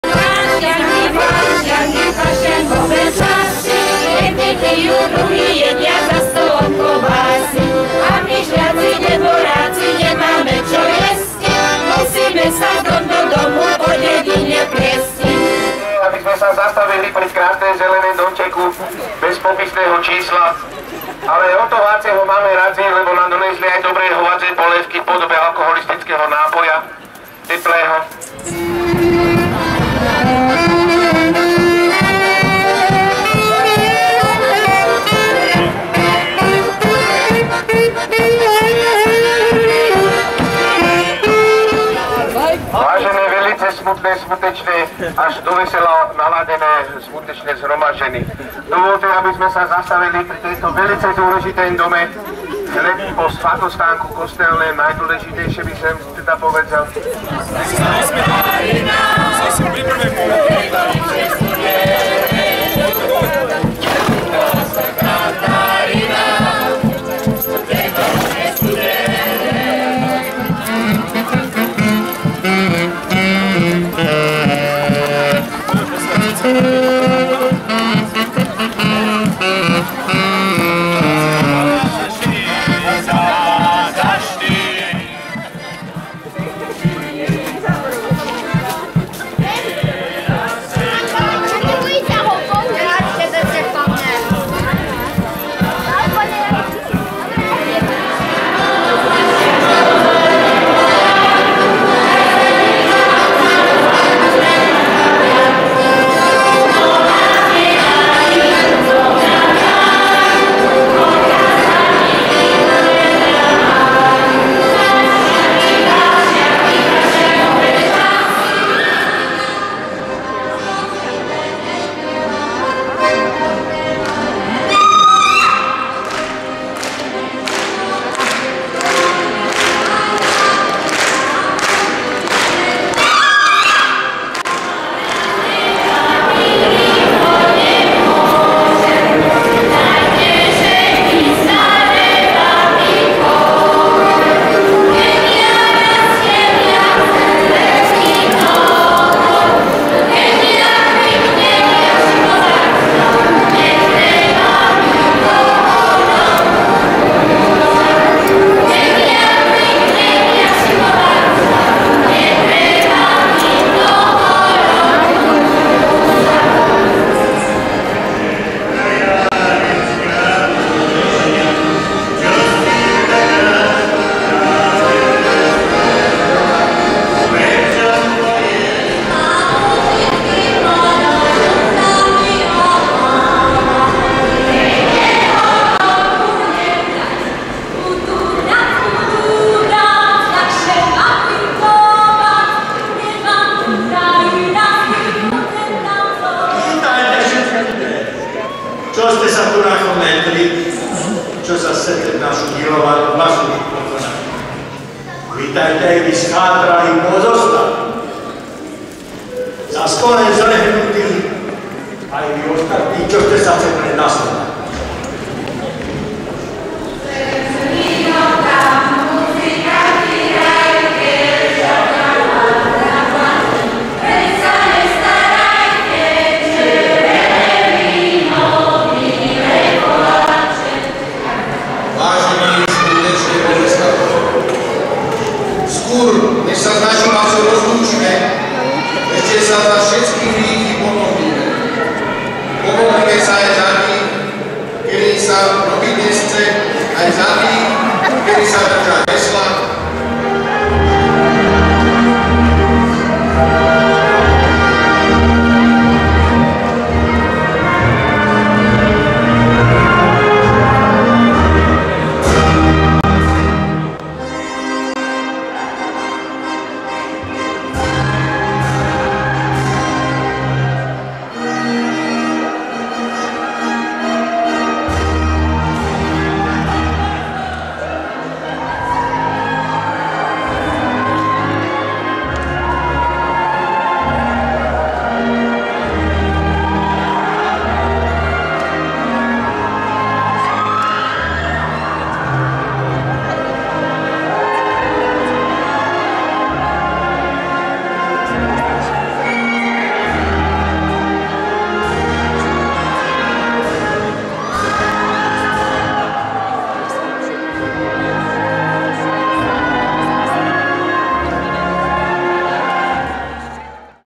Fasiangy Vinicne 2015 zostrih
Krátky zostrih videa Fašiangy 2015 v obci Viničné (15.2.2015)